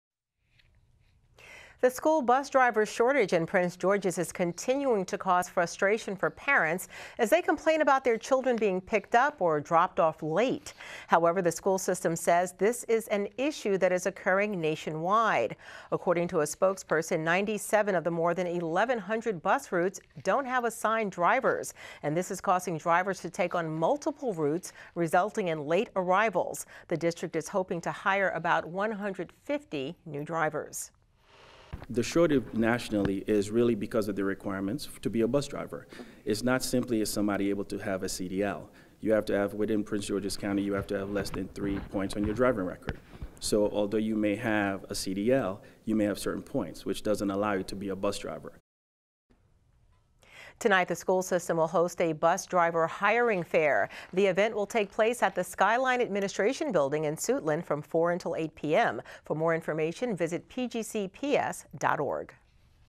PGCPS Bus Shortage
The Prince George's County School System discusses the bus driver shortage that has caused many students to arrive late to school.